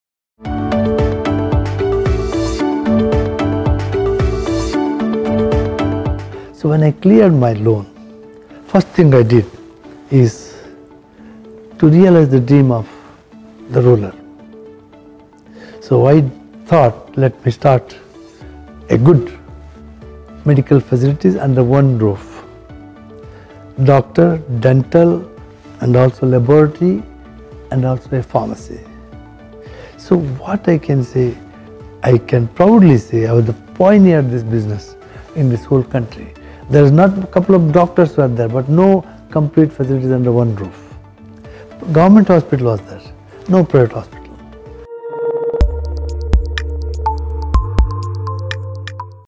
'Shetty Speaks' series - Establishing the first private healthcare facility in UAE
Establishing the first private healthcare facility in UAE: Dr. B. R. Shetty talks about his main motive in founding NMC.
‘Shetty Speaks’ series: In a first of its kind initiative, listen to Dr. B. R. Shetty speak about his experiences in life and business, core principles and philosophies he follows, his entrepreneurial instincts and advice to youngsters.